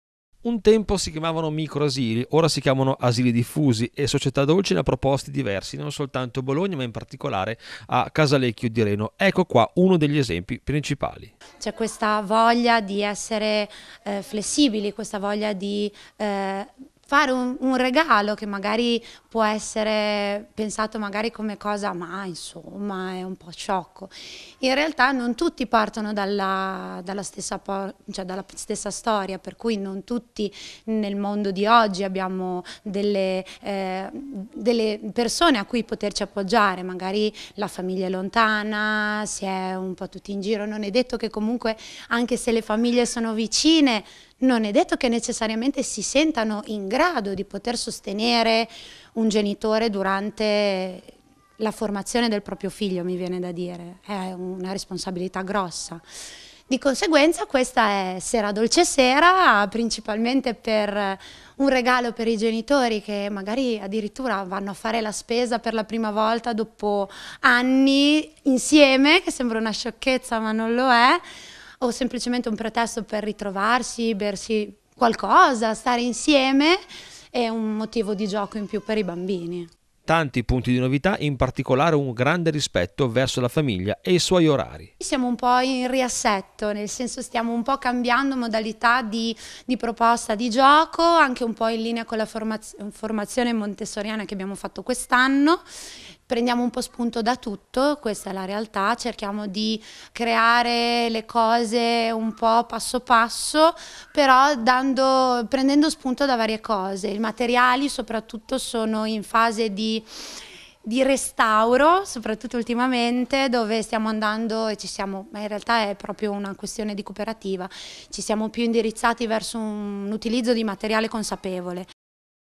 SeraDolceSera 24/05/2017 - TRC
Una festa su misura per i bambini, una sera libera per i genitori.
Apertura straordinaria fino alle 21.30, dei servizi 06 di Società Dolce a Casalecchio di Reno, Valsamoggia e Zola Predosa